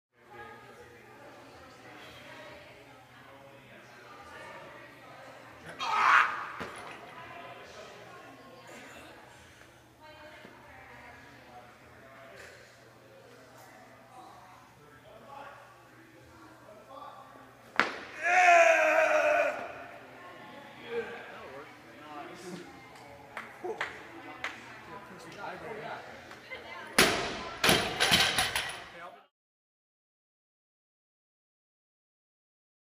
Jordan PR 120kg Clean and Jerk @ 64kg
Jordan Price of ABG does a PR Clean and Jerk, with a slight press out of 120kg @ 64kg, age 18
AverageBroz.com